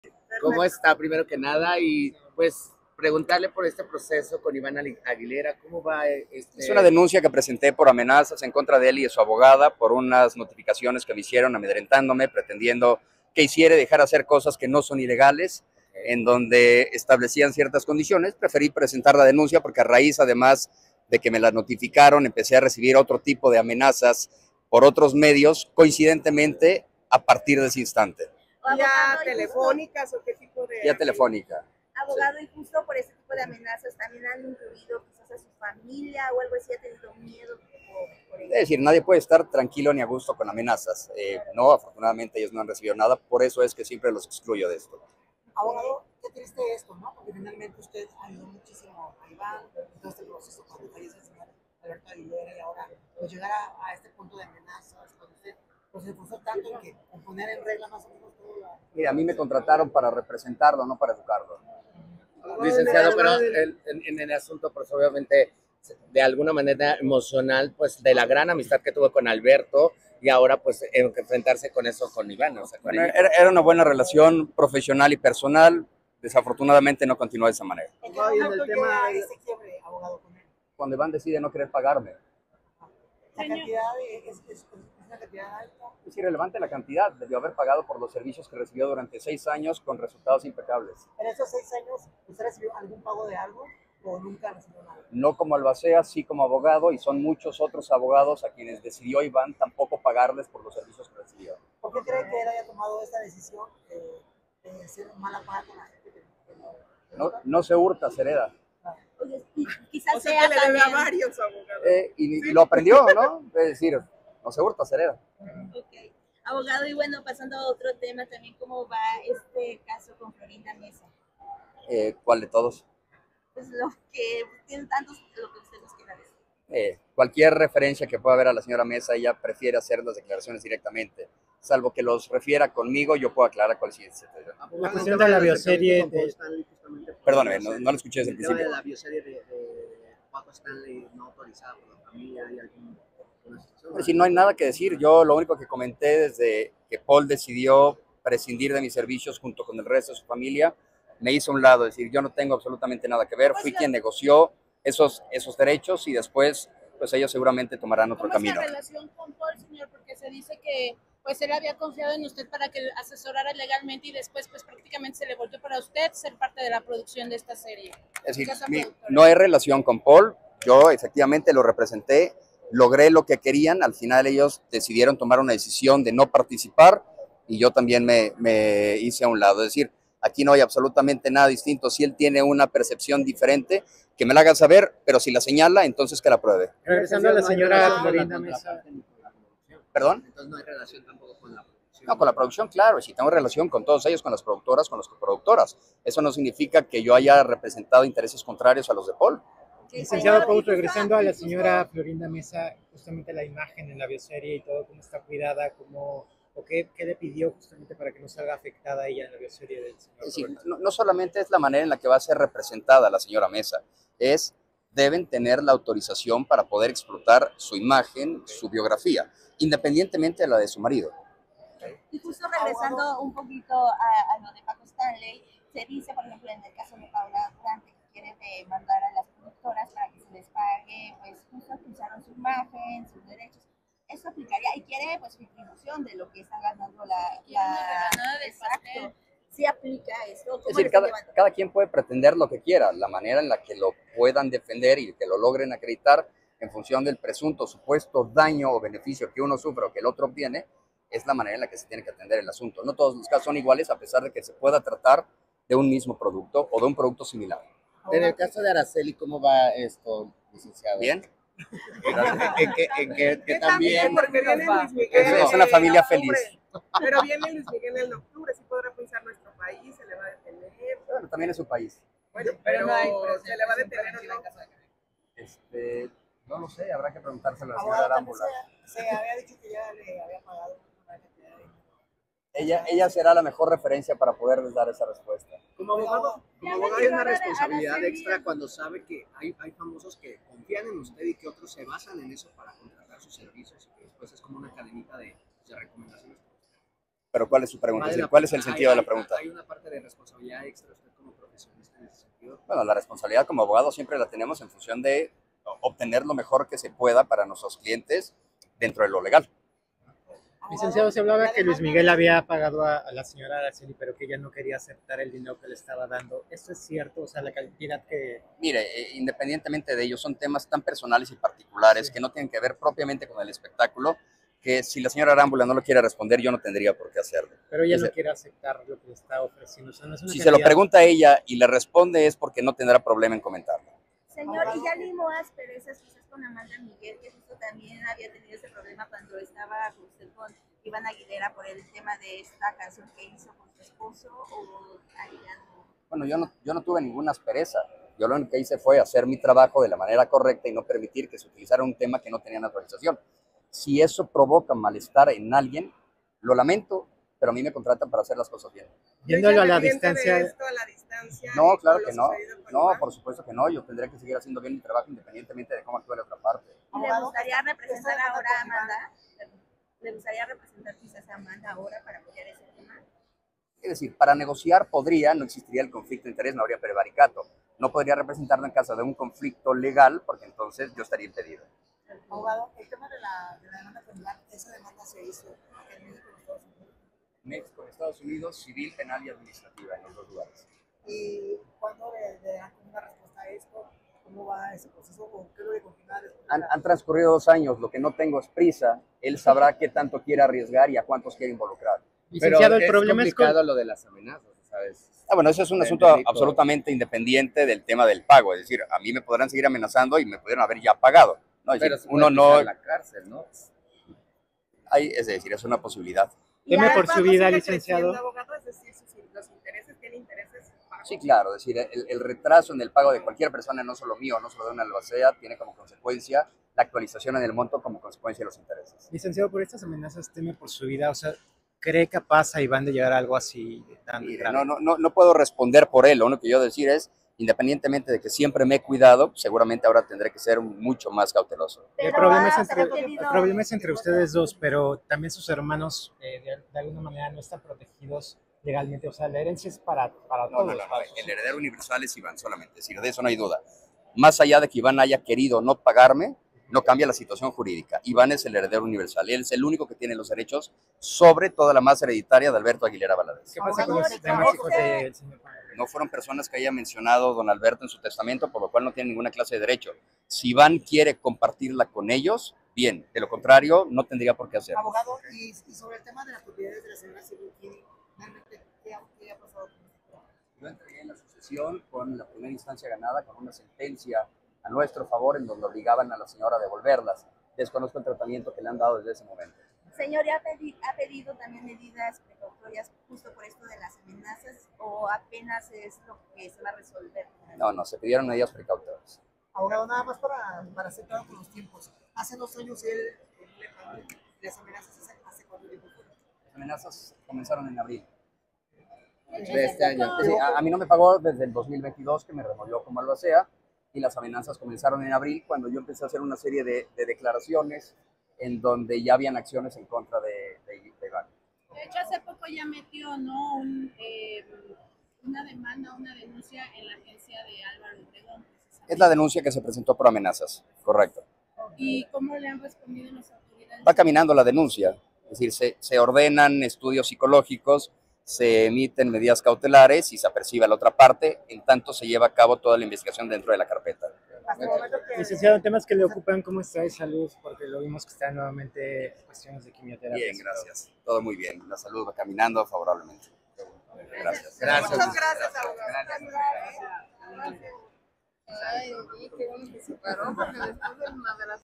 IVÁN AGUILERA TENDRÁ UNA DEMANDA POR AMENAZAS, NO SOY EL PRODUCTOR DE LA SERIE, GUILLERMO POUS.
El abogado Guillermo Pous advierte a Iván Aguilera que habrá demanda en su contra por amenazas hacia su persona y por falta de pagos.